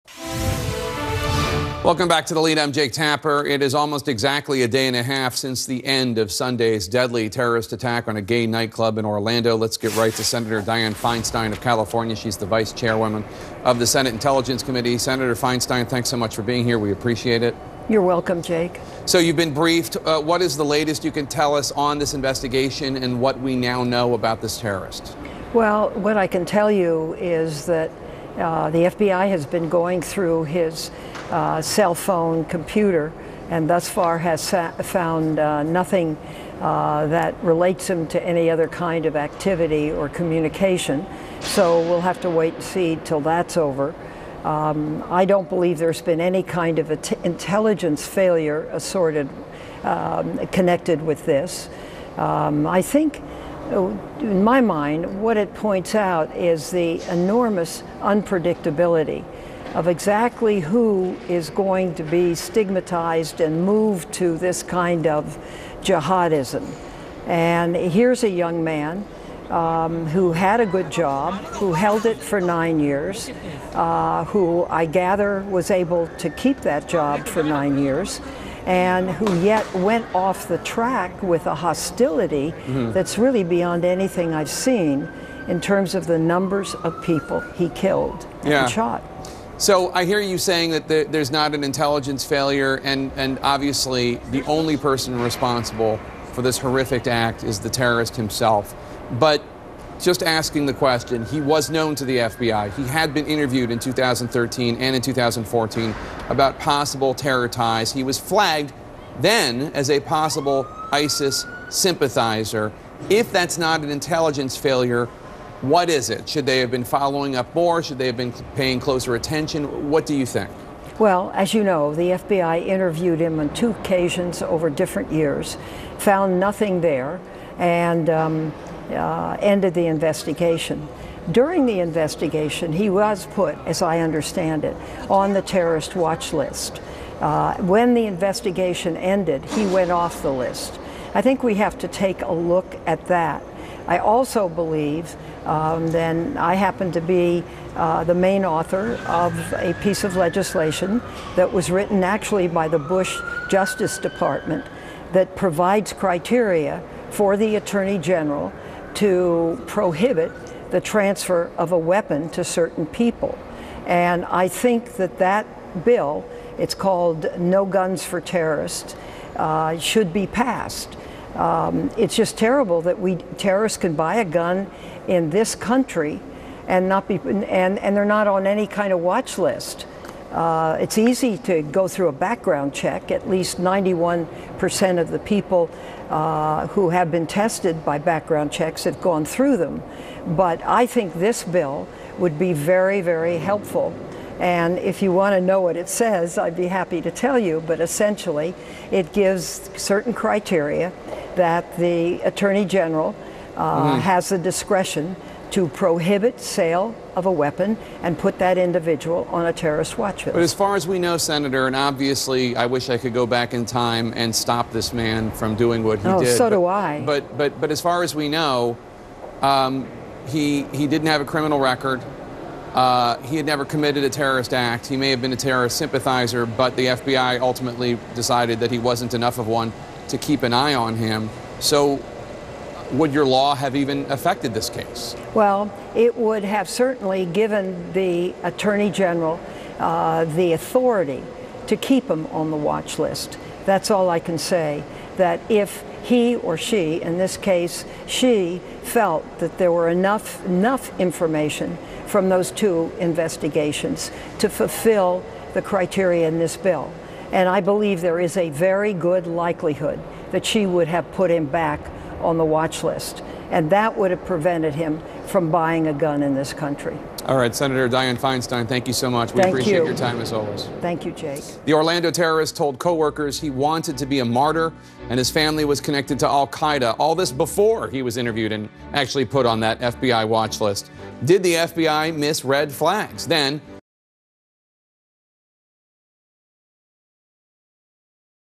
Senator Feinstein Refuses To Answer If Orlando Shooting Was An Intelligence Failure!
MOX NEWS NEEDS YOUR HELP TO CONTINUE!!